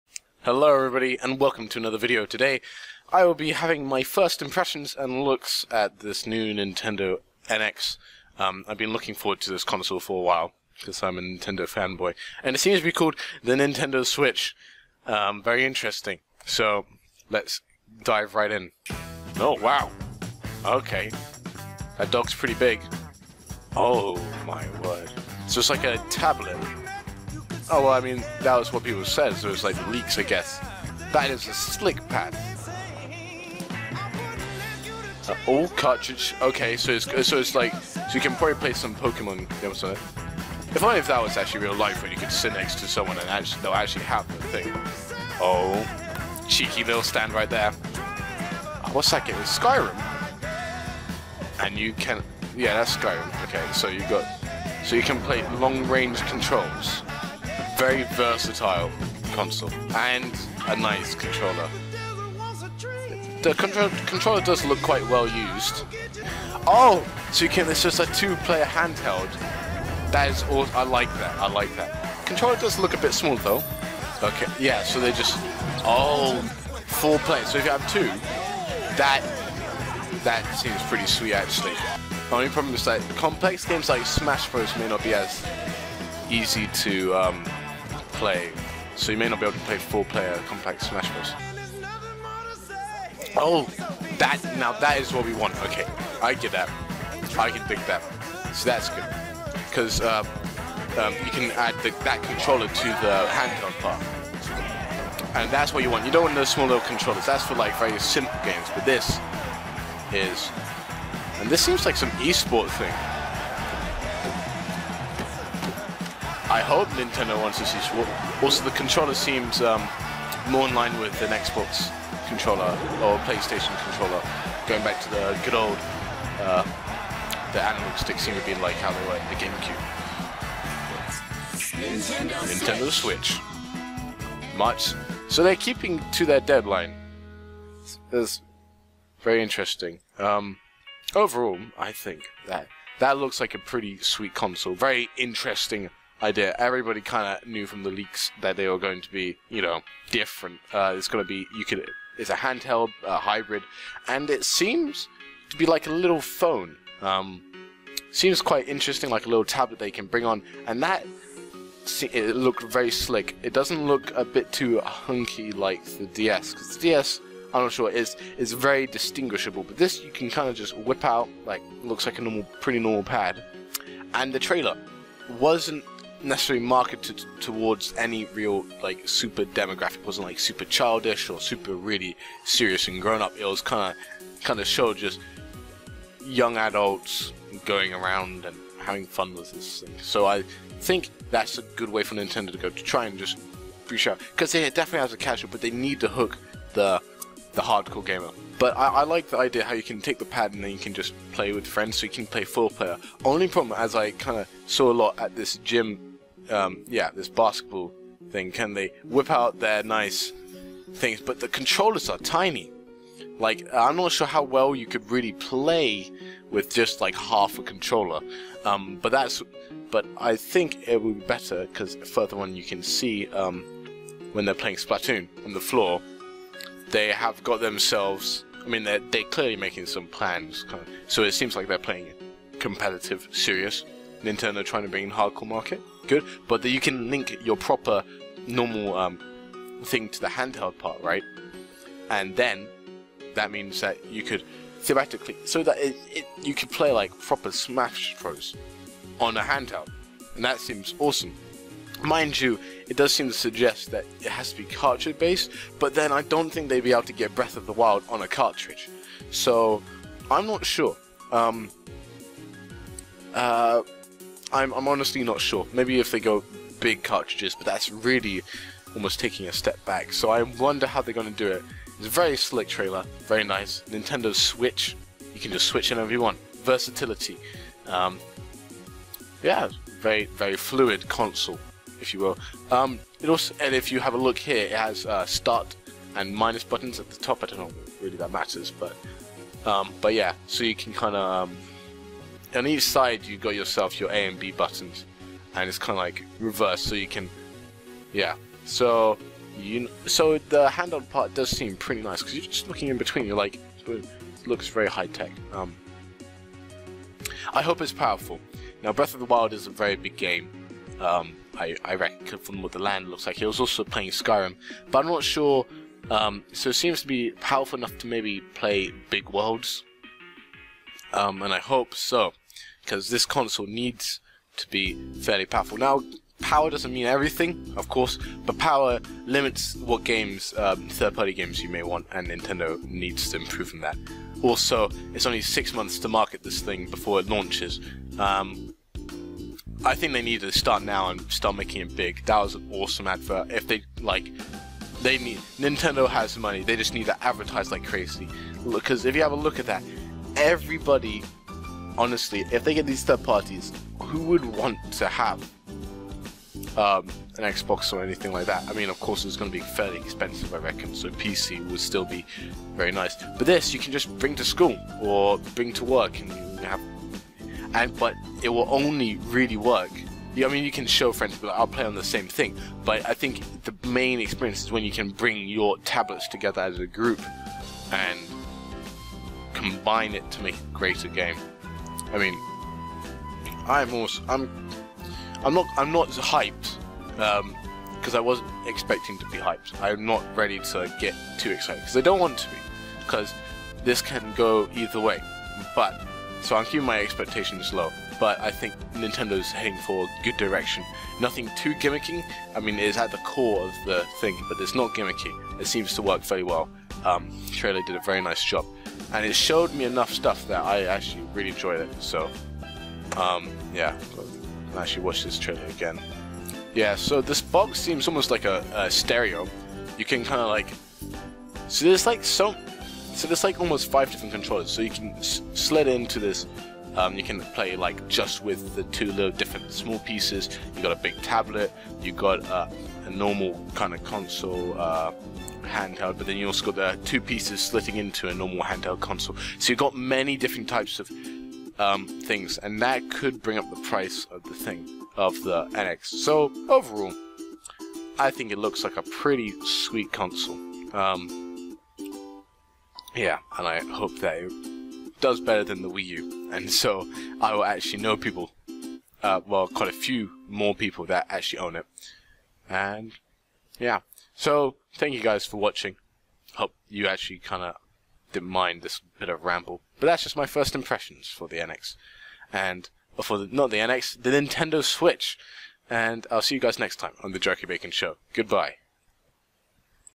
Nintendo Switch! Reaction and first impression!
ALL ABOARD THE HYPE TRAIN! CHOO CHOO!
The Nintendo Switch just dropped and here are my reactions and impressions.